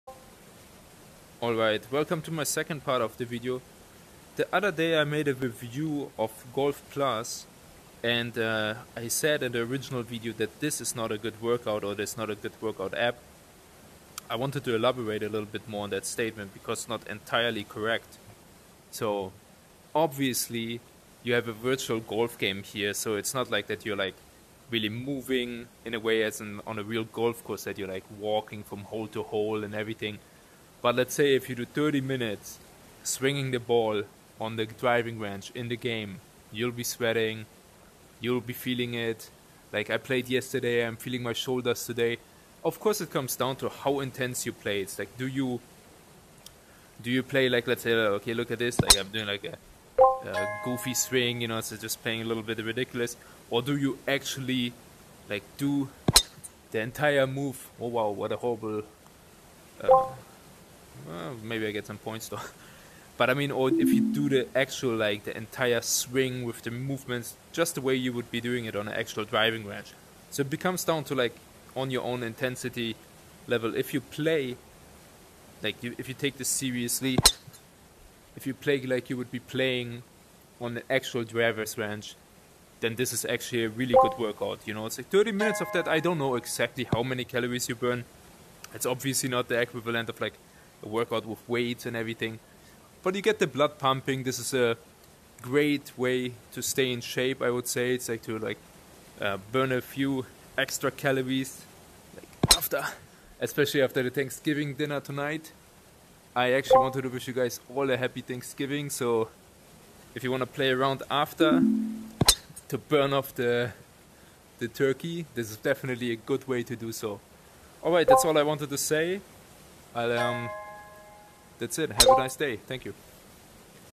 Is Golf+ on Oculus Quest 2 a Good VR Workout App?
Hi guys I wanted to elaborate on my original Golf + on Oculus Quest 2 Review from the other day. If you were wondering if this game is a good way to burn some extra calories make sure you check my video